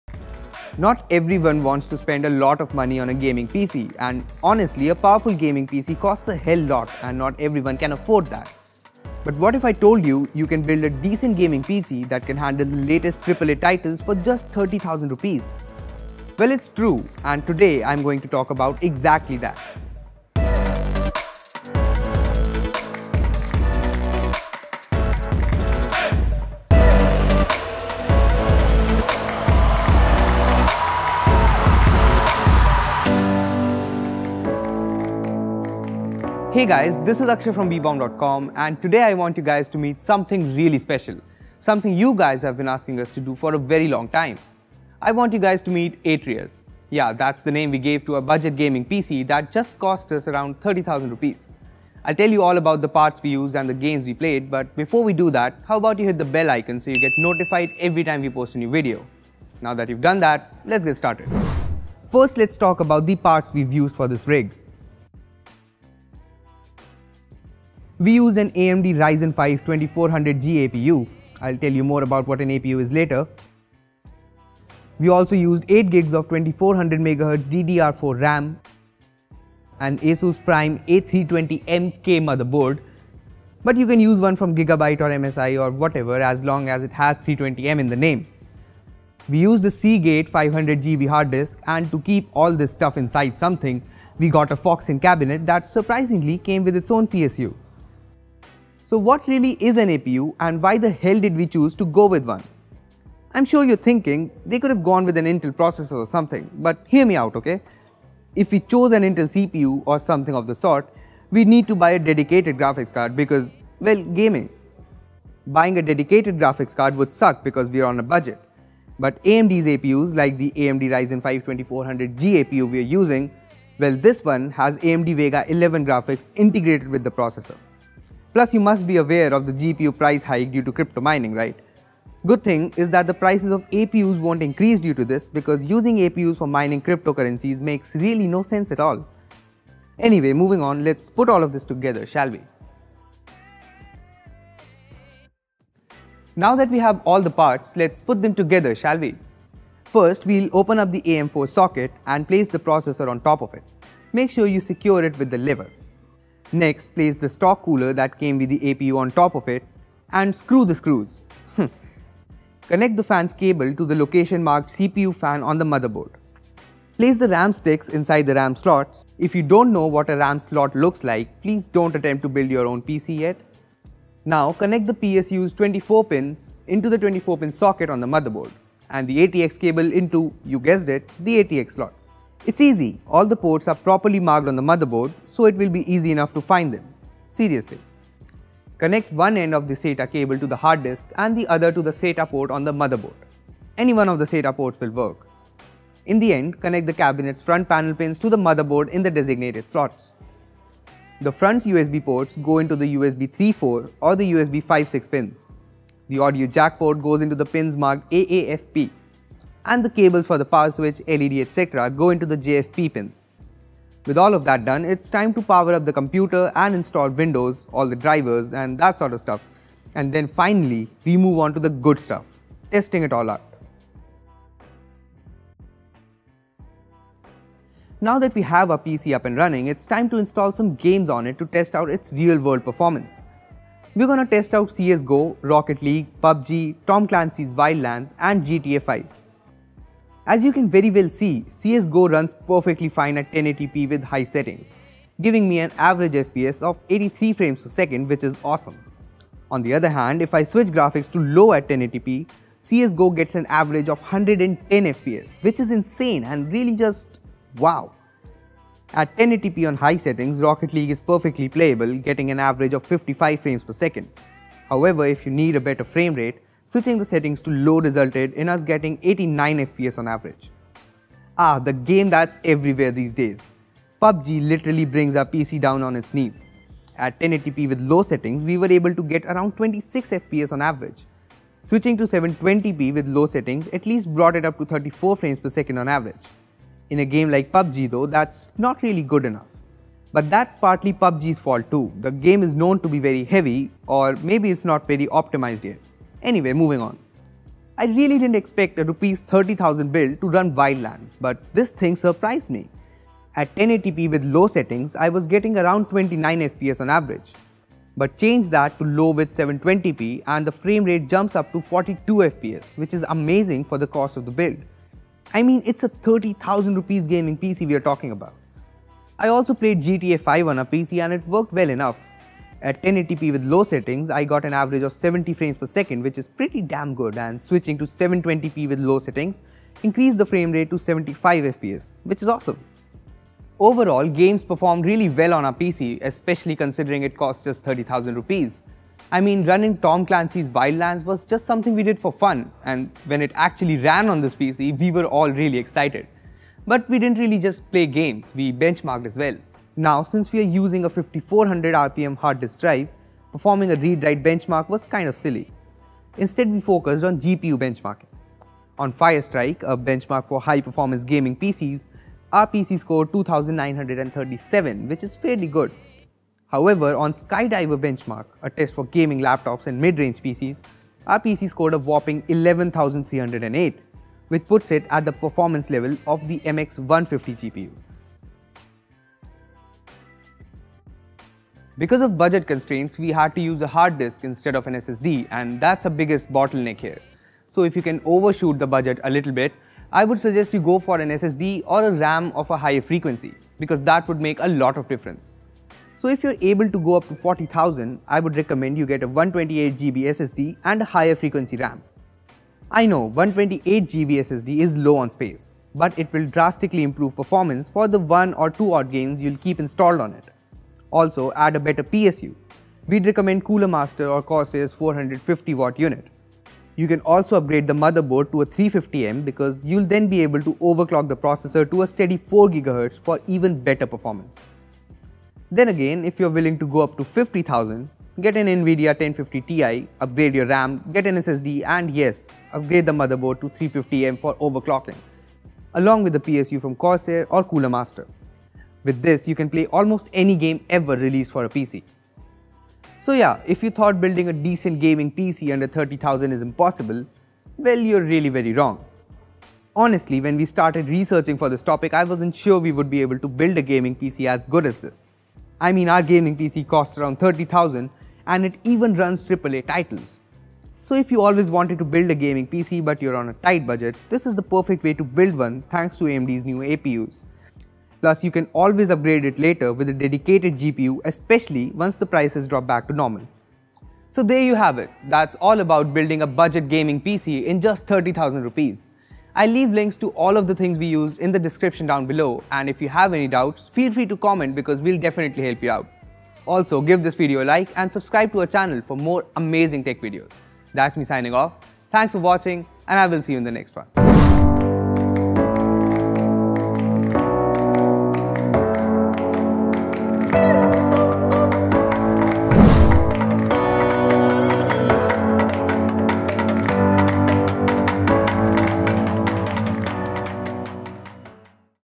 How to Build Budget Gaming PC in 30000 INR
There is a perception that gaming PCs cost a lot but that's not really true. You can build your own gaming PC for around ₹30000 aka $460. In this video, we should tell you how you can build your own budget gaming PC in around ₹30000 using AMD's new Ryzen 2400G APU.

The new Raven Ridge processor requires a BIOS update on the motherboard prior to installing the APU.

iBall Stella (Alternative cabinet with PSU)